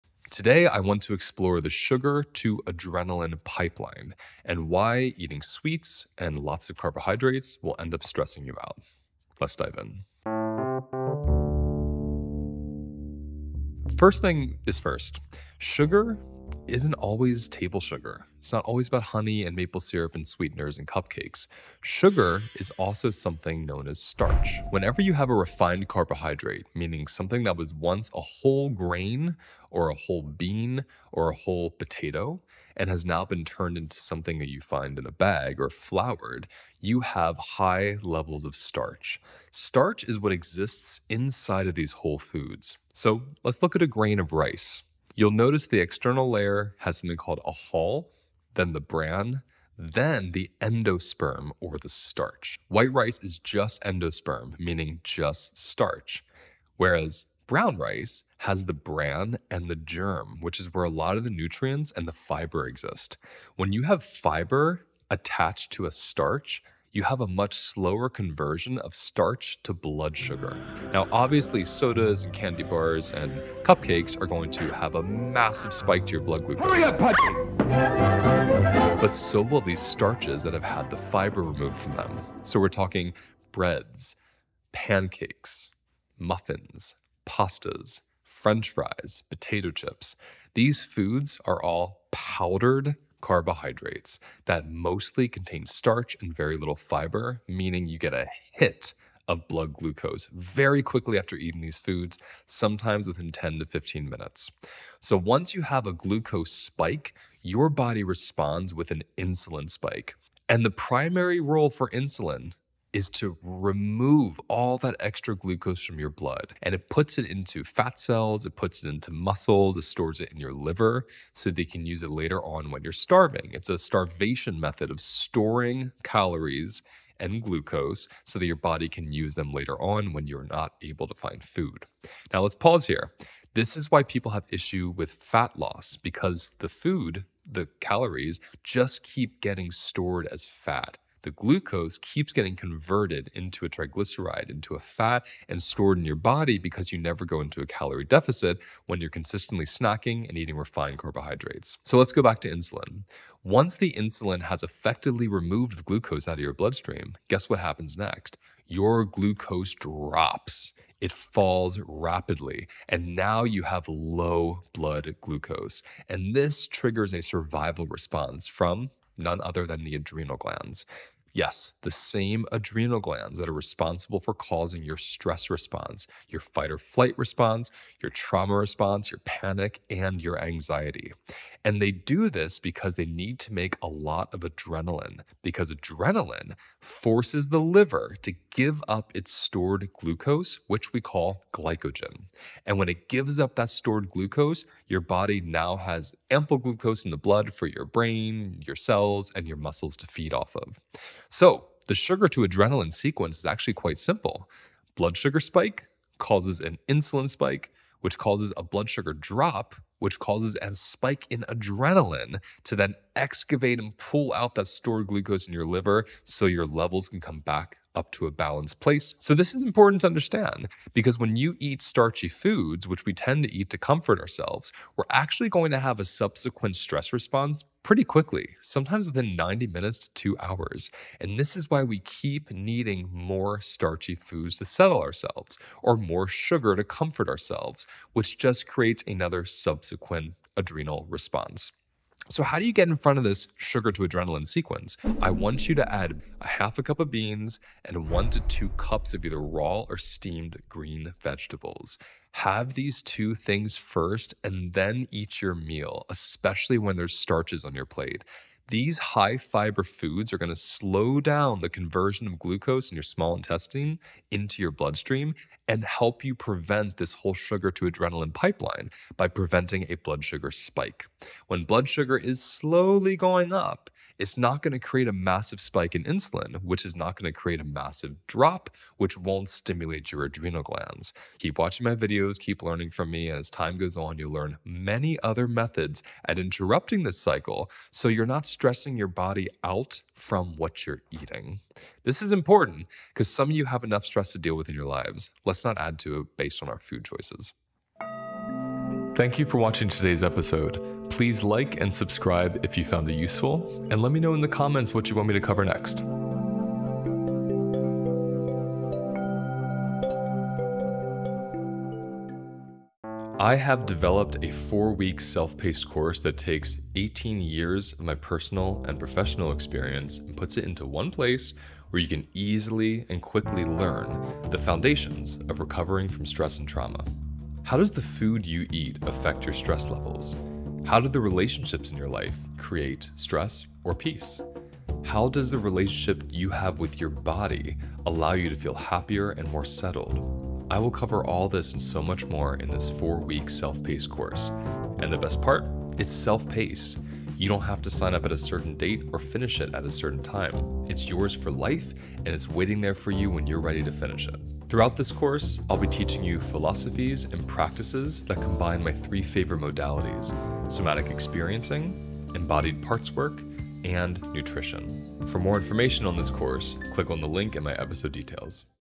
[Bonus Episode] How Sugar Leads To Adrenaline Spikes (More Stress)
Join Luis as he explores the effects of sugar, starch and refined carbohydrates on the body. Journey from the first bite to blood glucose levels spiking, all the way to the body's resulting stress response. Learn the balancing nutritional recommendations to employ before eating sugar, starch and refined carbohydrates to help mitigate the effects of adrenaline in the body.